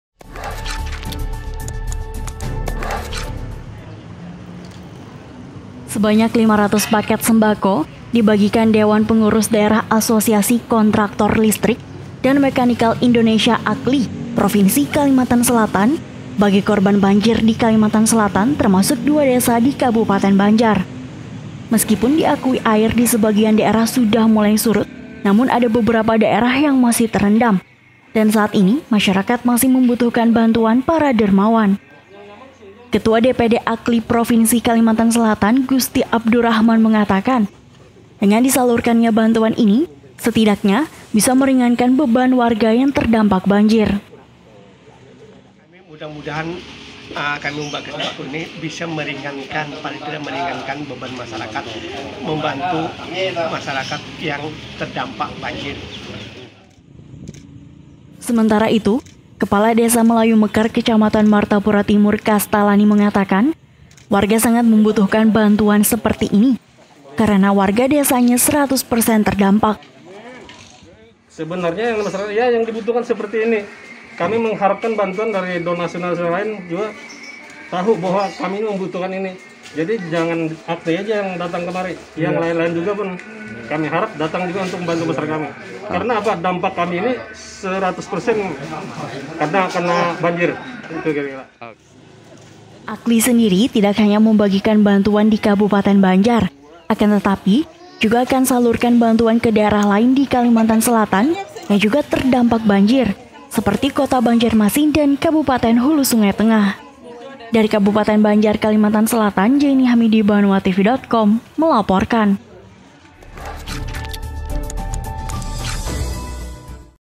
500 Paket Sembako Disalurkan DPD AKLI Kalsel Untuk Korban Terdampak Banjir
Bencana banjir yang melanda Provinsi Kalimantan Selatan, khususnya Kabupaten Banjar, mendapat banyak perhatian dari berbagai pihak, baik itu Pemerintah maupun swasta.

Kali ini giliran Dewan Perwakilan Daerah Asosiasi Kontraktr Listrik dan Mekanikal Indonesia (AKLI), curahkan perhatiannya kepada para korban banjir dengan membagikan bingkisan sembako.